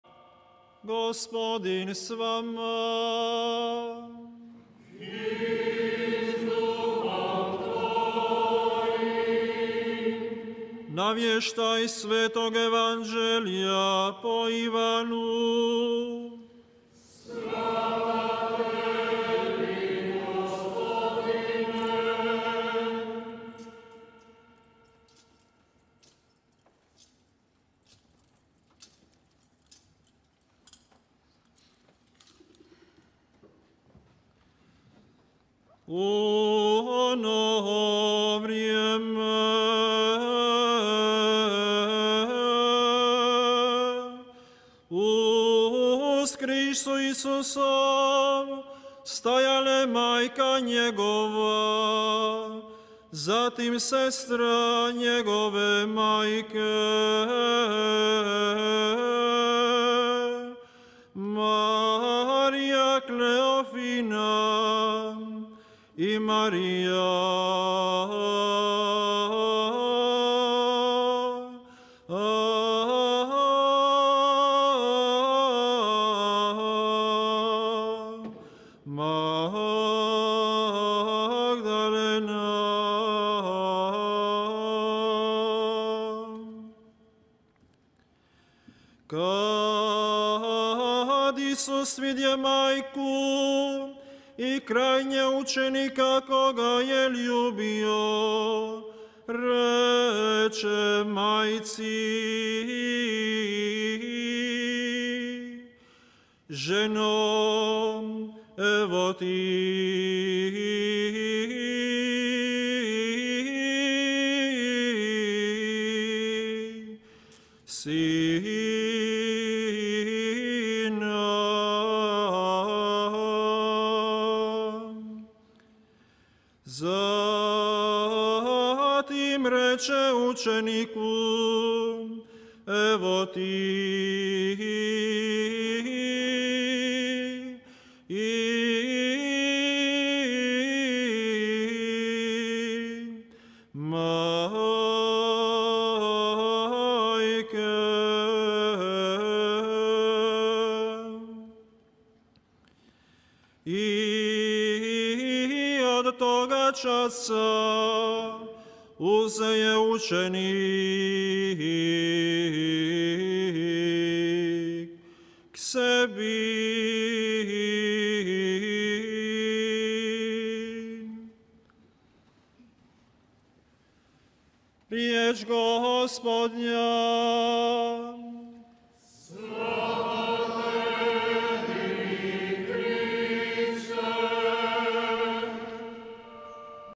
Evanđelje - napjev iz Hvara
Evanđelje pjeva don Jure, župnik u Postirima

Biskupsko ređenje mons. Ranka Vidovića, hvarskoga biskupa
Katedrala sv. Stjepana I., pape i mučenika, Hvar, 22. svibnja 2021.